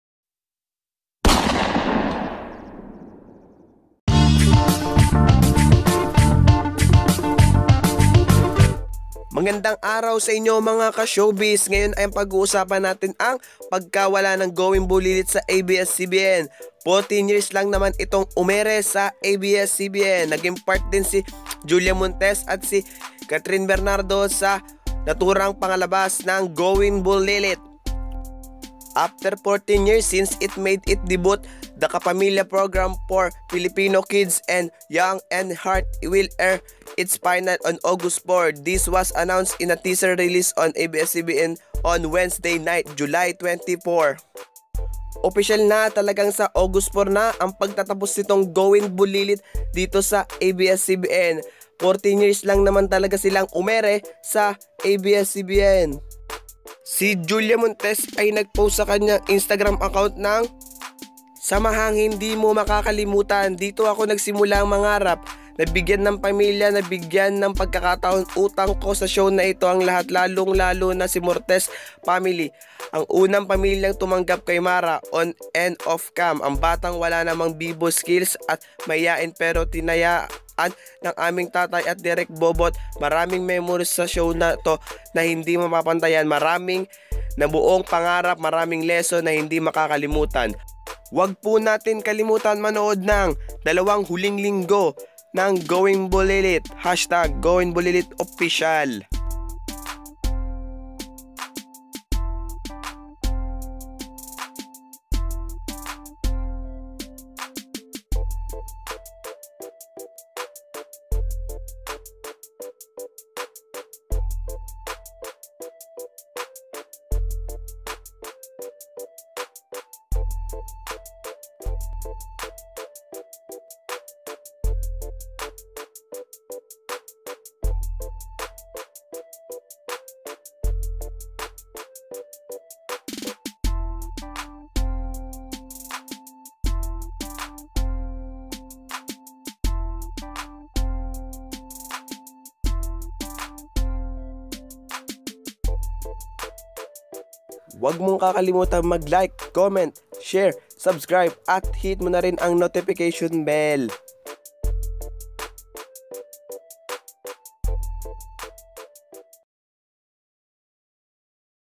Goin Bulilit OFFICIALLY Sa August 4, 2019 Ang PAGTATAPOS!
Malungkot pinapahayag ng abs-cbn huling dalawang linggo nalang ang goin bulilit 14 years naman itong umere sa abs-cbn 14 years on air ABS-CBN

Anong masasabi ukol dito ?
Mag commento sa baba!